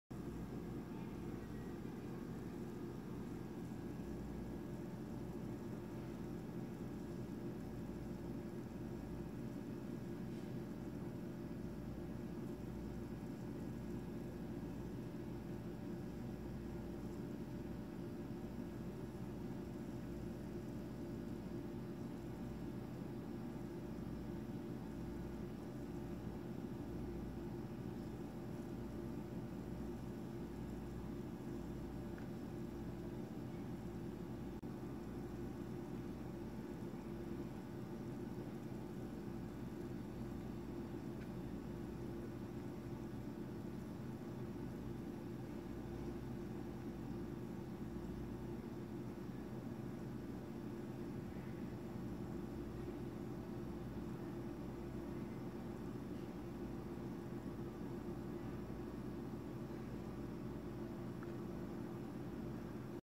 How to choose Protocol for your car on PX3 Vertical Screen Android Head Units
Only do this if the steering wheel control is not working.